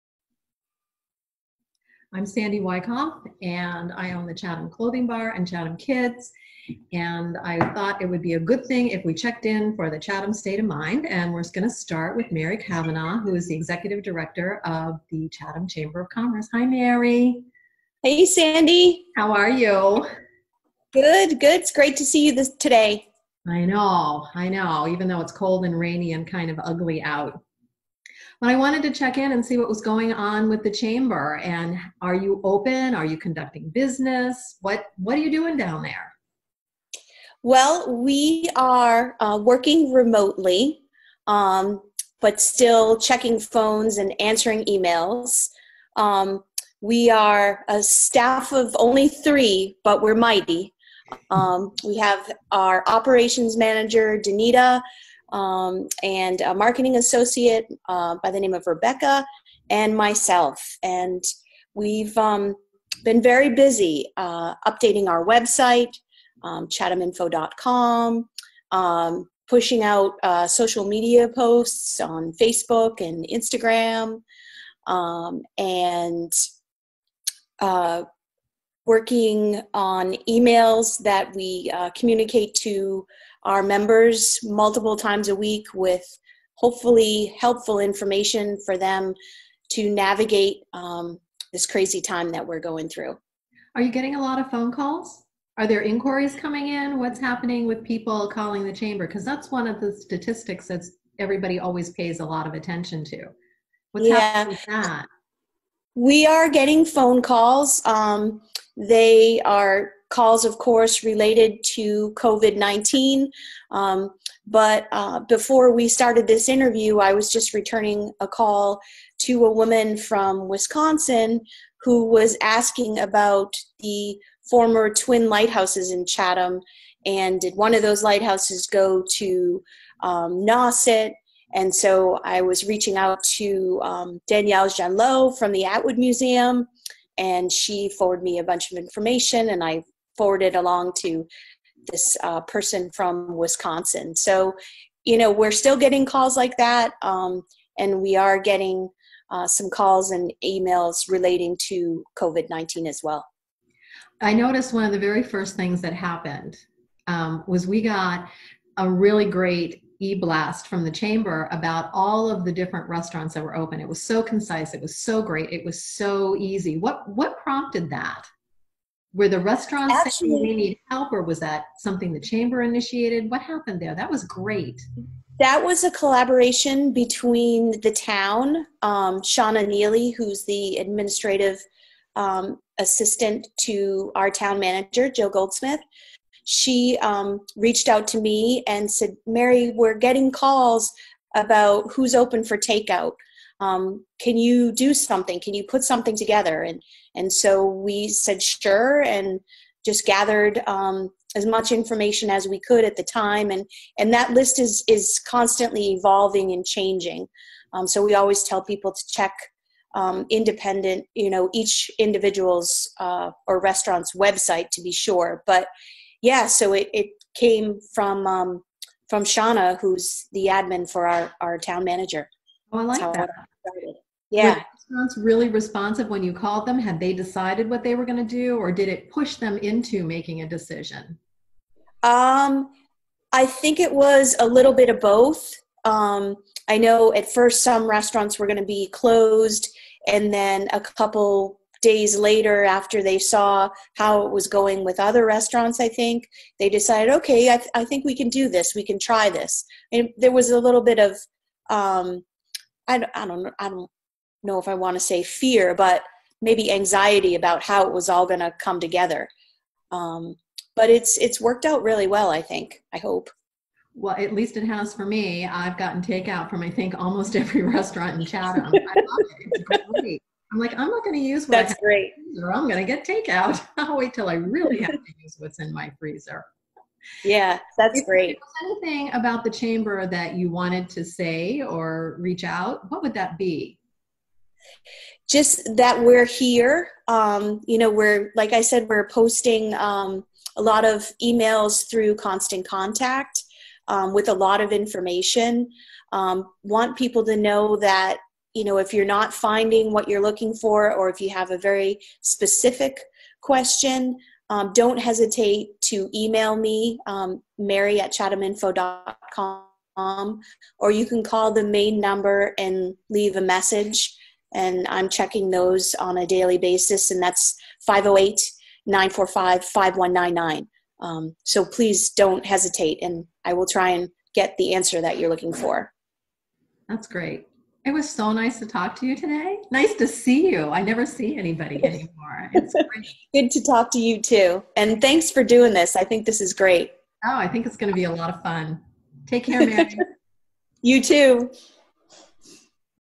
Chatham State Of Mind
Chatham State of Mind with Sandy Wycoff of Chatham Clothing Bar & Mary Cavanaugh of the Chatham Chamber of Commerce & Merchants Assoc.